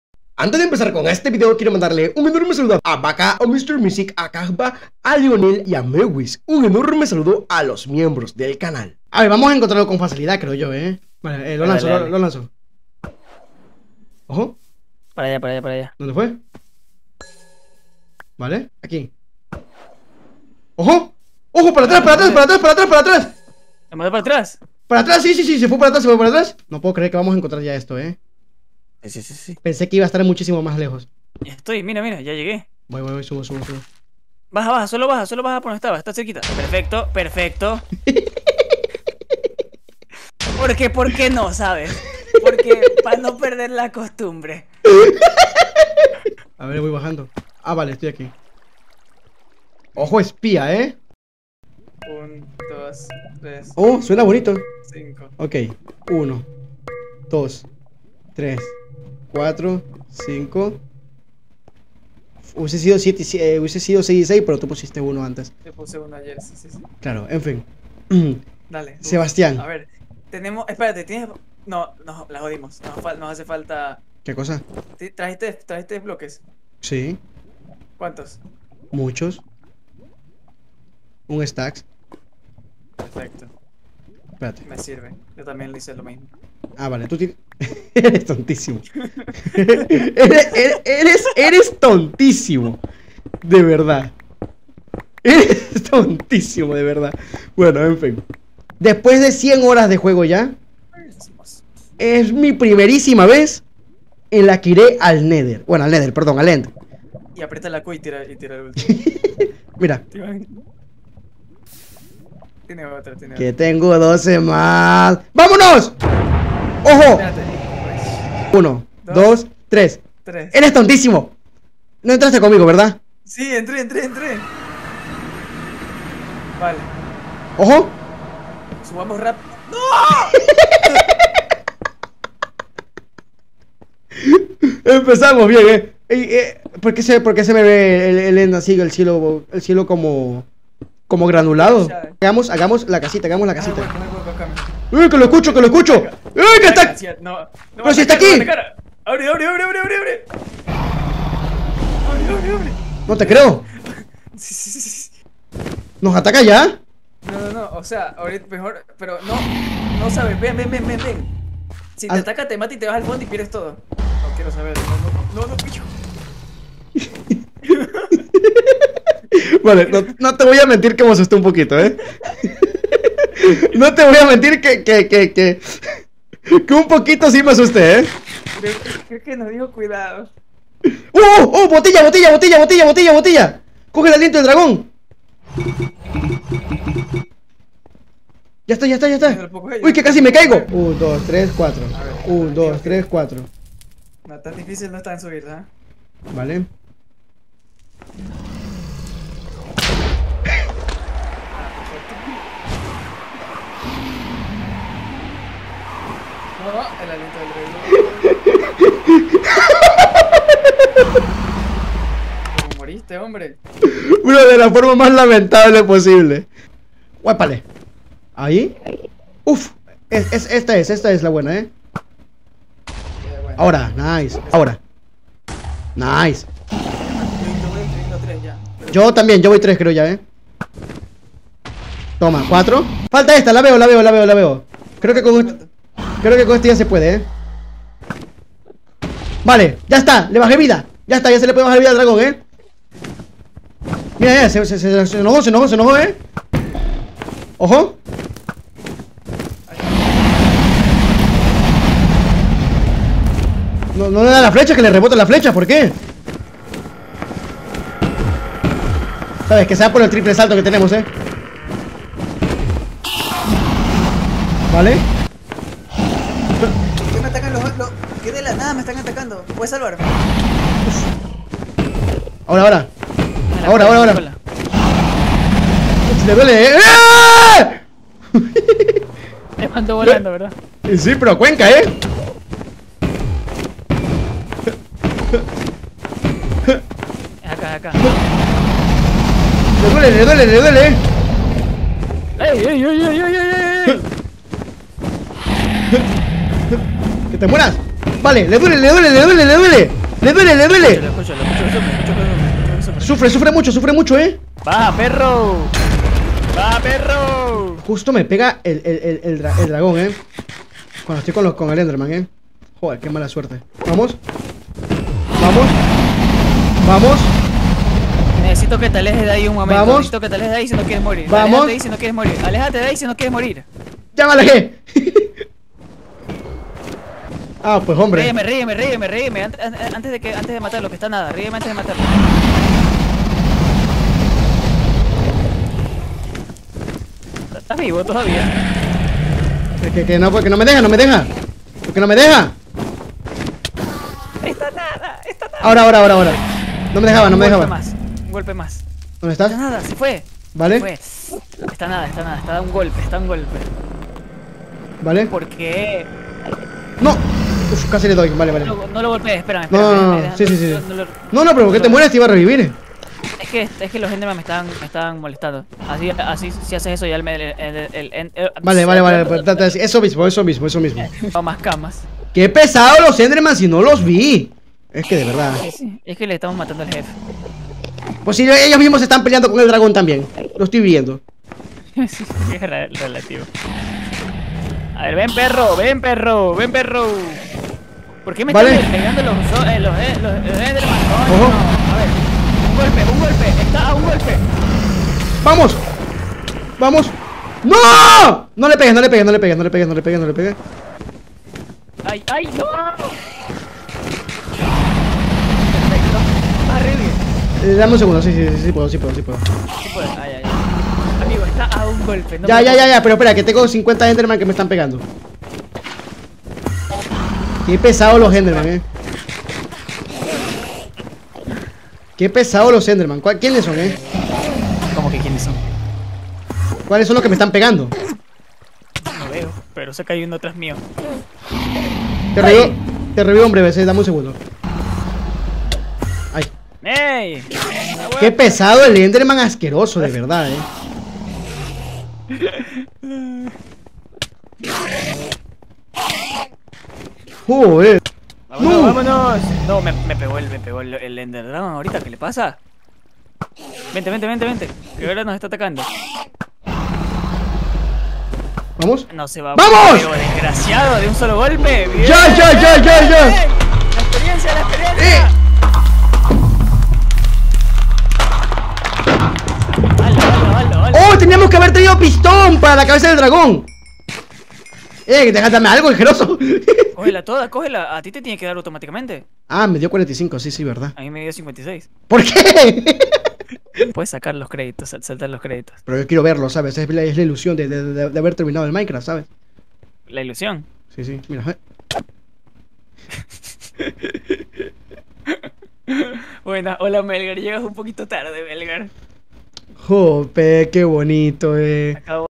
¡¡A POR EL DRAGON!! ¡¡MI PRIMERA VEZ EN EL END!! | BETALAND #21
▶️Quería agradecerles a todos por siempre estar apoyándome , Espero no manquear mucho hoy, y si es así pues... es algo inevitable 😆

⏺Con Dios en nuestra vida, todo irá bien.